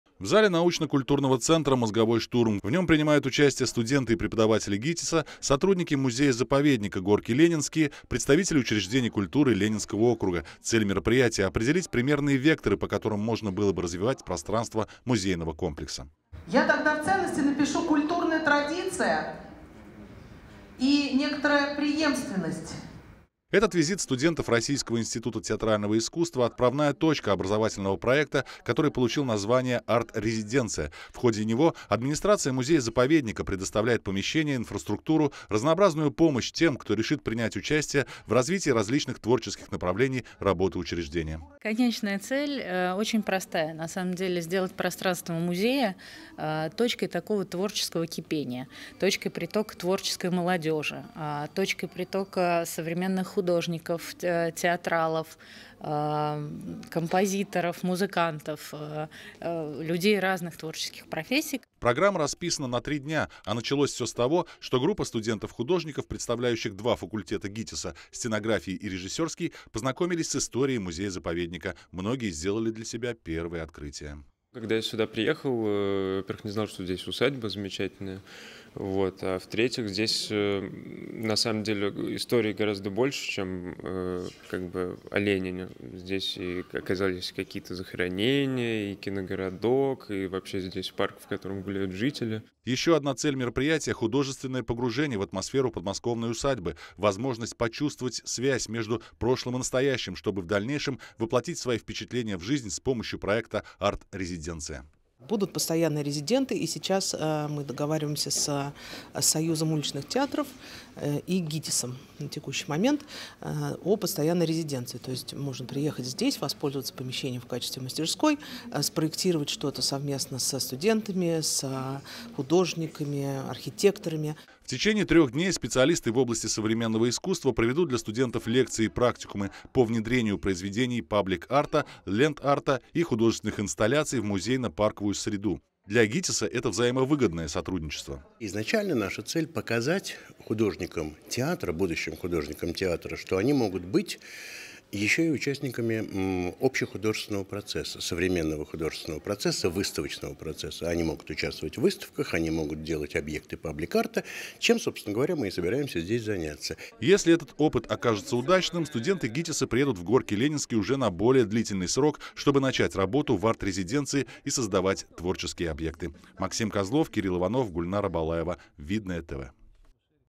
АРТ-РЕЗИДЕНЦИЯ
В музее-заповеднике «Горки Ленинские» стартовал образовательный проект, который получил название «Арт-резиденция». Первыми в нём приняли участие студенты Российского института театрального искусства. О сути нового проекта расскажет Максим Козлов.